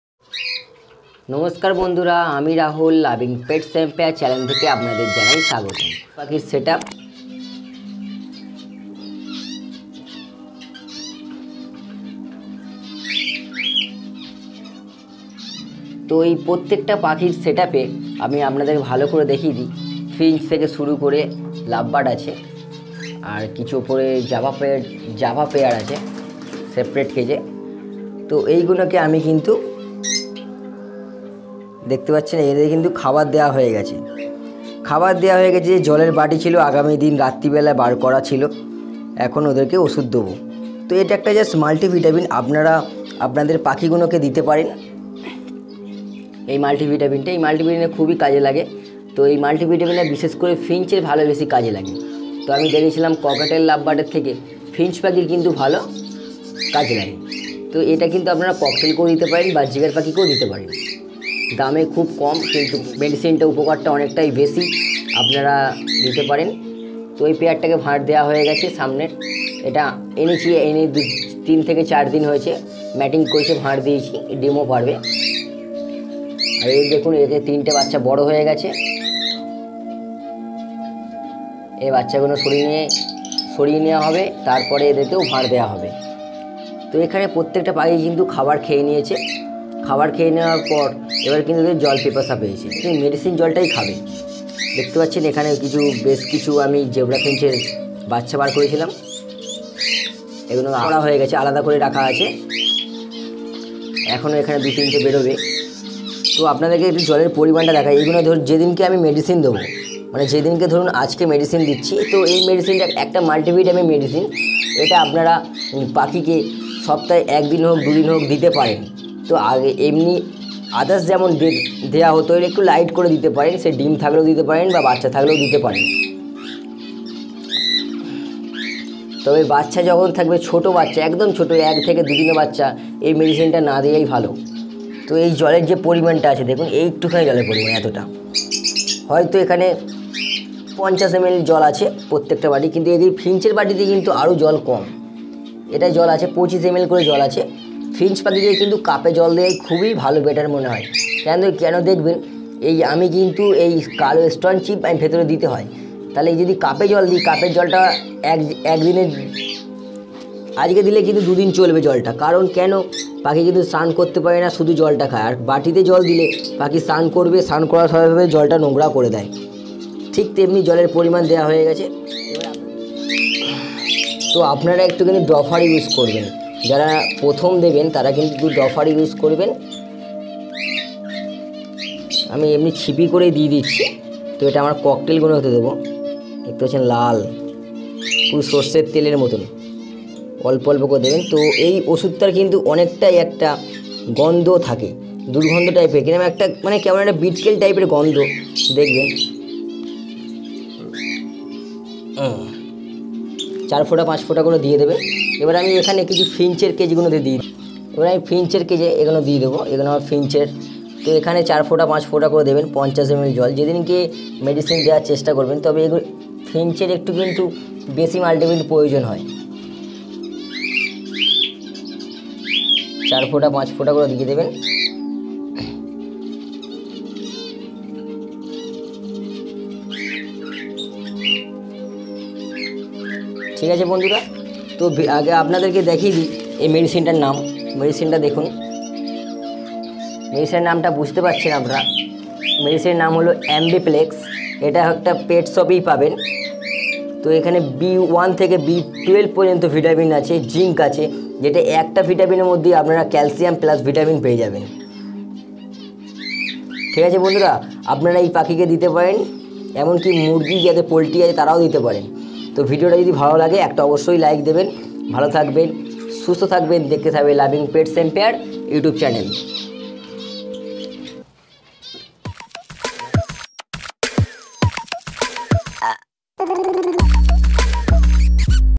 Best multivitamin for birds, পাখিদের জন্য ভালো ভিটামিন,
যে সকল পাখি পালন সাধারণত পাখি পেলে থাকেন
বারজিগার, জেব্রা ফিঞ্চ পাখি, লাভ বার্ড ও ককটেল পাখি,এই সব ধরনের পাখিকে আপনারা এই মাল্টিভিটামিন টা খুবই ব্যবহার করতে পারেন মাল্টিভিটামিন দামে কম হলেও উপকারিতা অনেক বেশি হয়ে থাকে আমি আমার বাজিগর পাখি সেটা ব্যবহার করেছি ও ভালো সাফল্য পেয়েছি তাই আজকে আপনাদের সাথে শেয়ার করছি ভিডিওটা যদি ভালো লাগে একটা অবশ্যই লাইক দেবেন ভিডিওটা অবশ্যই দেখবেন তবে বুঝতে পারবেন
ধন্যবাদ